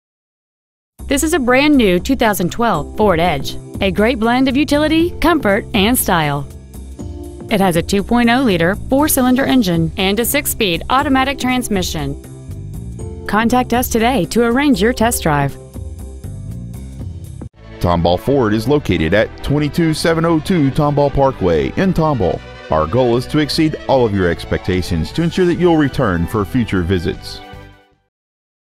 2012 Ford Edge Tomball TX
Year : 2012

 Make : Ford

 Model : Edge

 Engine : 2.0L Ecoboost I4

 Trans . : Automatic

 Exterior : White Platinum Tri-Coat Metallic

 Miles : 4

 Interior : HTD LEATHER TRIMMED SEATS SIENNA

 Stock : BA69385

 Tomball Ford

 22702 Tomball Parkway